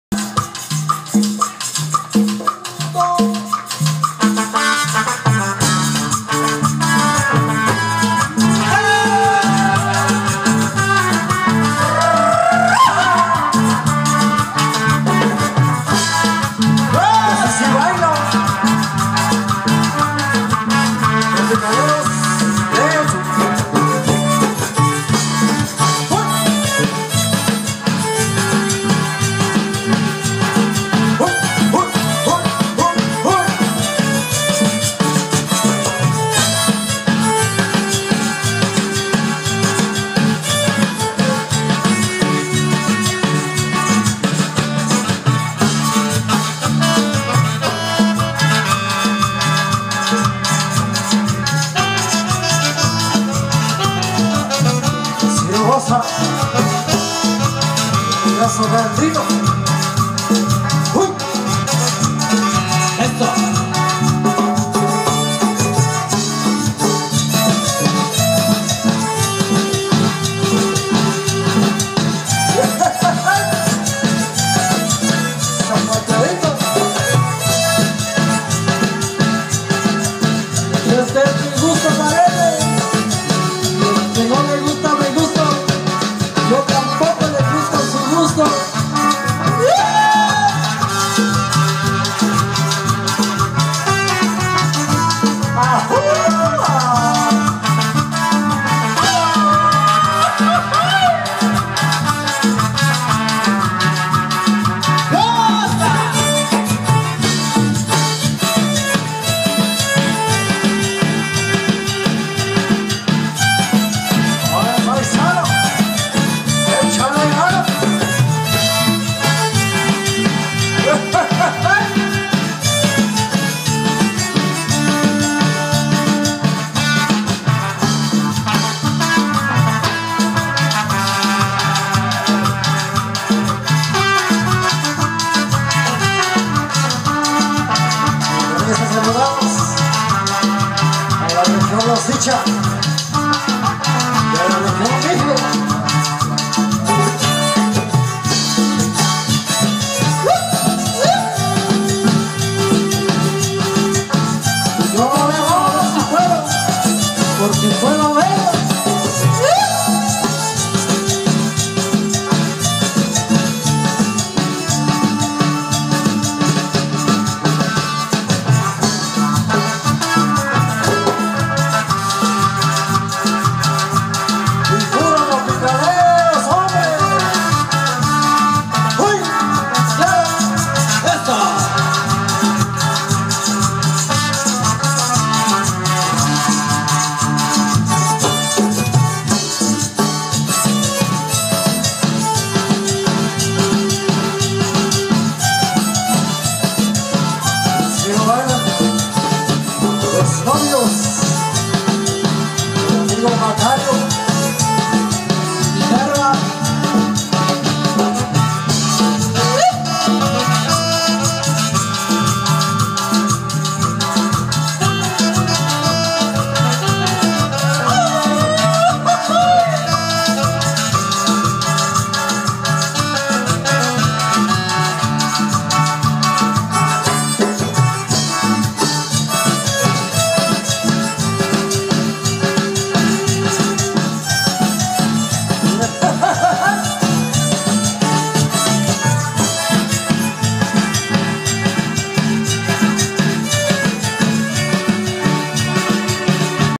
GRUPO LOS TROPICALEROS DEL SUR. 27 De Mayo 2017 En Miahuatlan De Porfirio Diaz Oaxaca
Grupo Los Tropicaleros Del Sur. 27 de mayo 2017 en miahuatlan de porfirio diaz oaxaca para contrataciones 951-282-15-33 / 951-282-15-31 / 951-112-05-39.